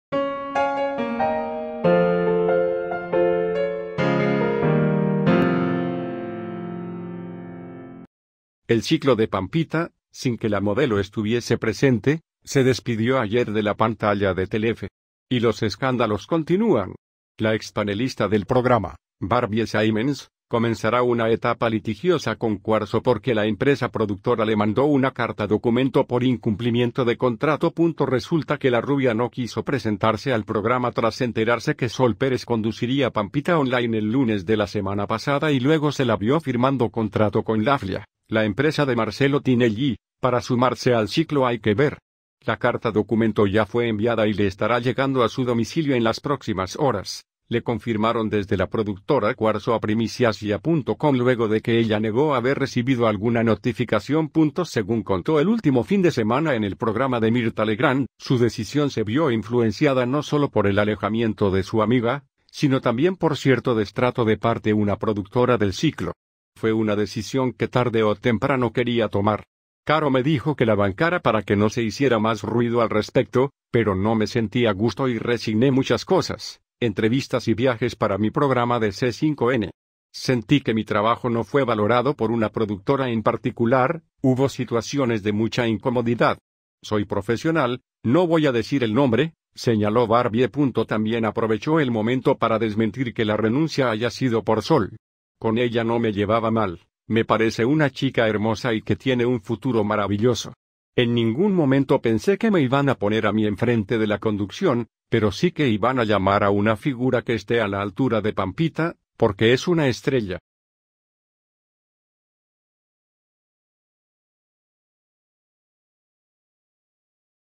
Problema judicial
El ciclo de Pampita, sin que la modelo estuviese presente, se despidió ayer de la pantalla de Telefe. Y los escándalos continúan. La expanelista del programa, Barbie Simons, comenzará una etapa litigiosa con Kuarzo porque la empresa productora le mandó una carta documento por incumplimiento de contrato.Resulta que la rubia no quiso presentarse al programa tras enterarse que Sol Pérez conduciría "Pampita Online" el lunes de la semana pasada y luego se la vio firmando contrato con "Laflia", la empresa de Marcelo Tinelli, para sumarse al ciclo "Hay que ver"."La Carta Documento ya fue enviada y le estará llegando a su domicilio en las próximas horas", le confirmaron desde la productora Kuarzo a PrimiciasYa.com luego de que ella negó haber recibido alguna notificación.Según contó el último fin de semana en el programa de Mirtha Legrand, su decisión se vio influenciada no solo por el alejamiento de su amiga, sino también por cierto "destrato" de parte una productora del ciclo."Fue una decisión que tarde o temprano quería tomar. Caro me dijo que la bancara para que no se hiciera más ruido al respecto, pero no me sentí a gusto y resigné muchas cosas, entrevistas y viajes para mi programa de C5N. Sentí que mi trabajo no fue valorado por una productora en particular, hubo situaciones de mucha incomodidad. Soy profesional, no voy a decir el nombre", señaló Barbie.También aprovechó el momento para desmentir que la renuncia haya sido por Sol. "Con ella no me llevaba mal, me parece una chica hermosa y que tiene un futuro maravilloso. En ningún momento pensé que me iban a poner a mi enfrente de la conducción, pero sí que iban a llamar a una figura que esté a la altura de Pampita, porque es una estrella".